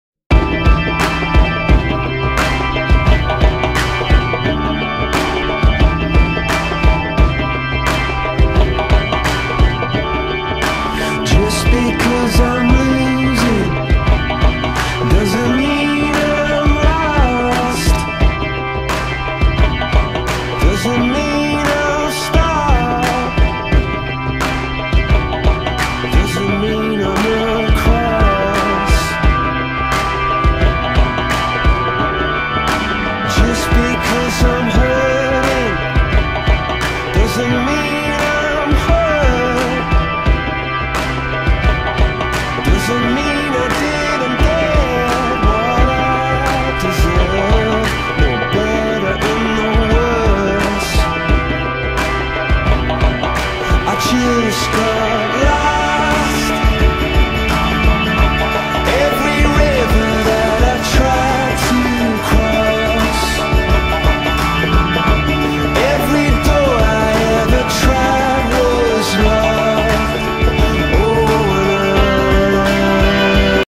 coldplay stop motion
another bad stop motion. it doesnt really mean anything its just a salt and pepper shaker and a teddy bear following barbie shoes